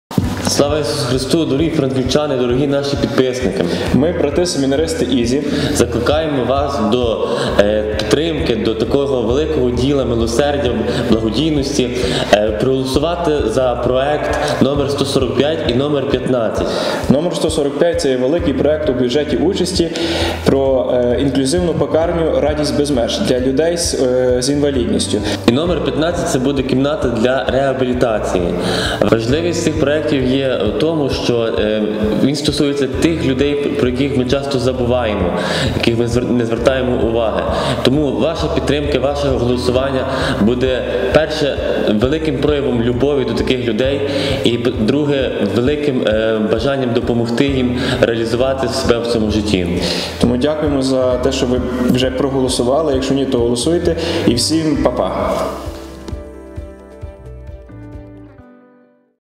Підтримаймо інклюзивні проекти № 145 та №15
Дорогі друзі! Нам і надалі потрібна ВАША ПІДТРИМКА у голосуванні за проекти № 145- для облаштування інклюзивної пекарні та № 15 - від Навчально-реабілітаційного центру.
"Ці діти, які часто не мають місця в суспільстві. Які після закінчення школи приречені жити в самотності...
Ці дитячі душі, що живуть в уже дорослих тілах і по людськи прагнуть спілкування і бути корисними.
 Ці діти, яким так украй необхідно зуміти повірити в себе, у те, що їх зусилля і боротьба не є марними, а праця - потрібною.
І їх мами, які, як і кожна з нас, так хочуть пишатися своїм дитям...
Унікальний проект - пекарня, де працюватимуть особи (а по суті своїй - діти) з обмеженими можливостями, місце, де їх праця і їх зусилля знайдуть можливість реалізуватися і стати частинкою різнобарвної мозаїки нашого життя. Місце, яке дасть їм можливість відчувати себе корисними і потрібними, можливість здобувати вміння і вчитися пишатися своїми досягненнями. Місце, яке стане острівцем доброти і надії у нашому місті для тих, які, можливо, уже її втратили...
   Допоможіть у облаштуванні нашої пекарні, проголосуйте за проекти  145 та 15 в бюджеті участі 2021.
 Початок уже покладено. Є певний досвід залучення дітей до праці, але масштаби можуть і повинні бути більшими, мають бути можливості для більшої кількості осіб, які здатні працювати під проводом асистентів.
   Тож просимо, дайте надію тим, які без нашої допомоги просто  залишаться самотніми у цьому світі. І хай кожному знайдеться місце серед нас".